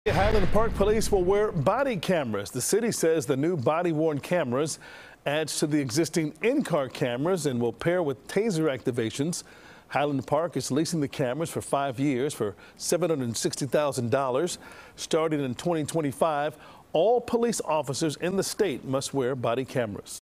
Highland Park police will begin to wear body cameras
The city says the new body-worn cameras add to the existing in-car cameras and will pair with taser activations.